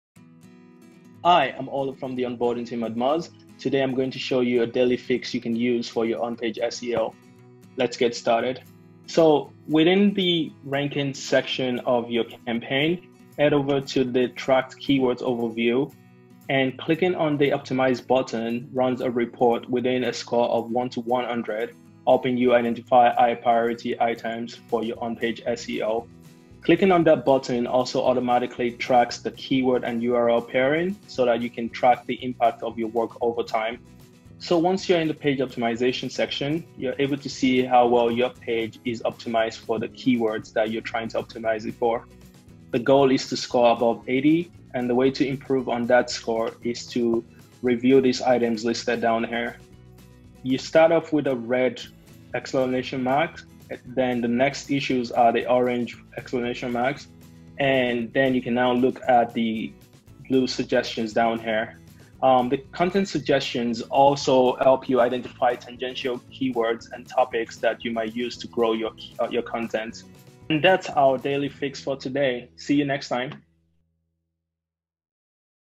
Daily SEO Fix - On-page optimization
There aren’t many things more confusing than seeing pages rank for keywords that have absolutely nothing to do with your business. You're always signaling something to the search engines — whether you intend to or not. Optimizing your on-page SEO ensures you control that signal.

On-page SEO is the practice of optimizing individual web pages for a specific keyword(s) in order to rank higher and earn more relevant traffic in search engines.

In this Daily Fix, I show you how to use the page optimization tool to improve your on-page SEO.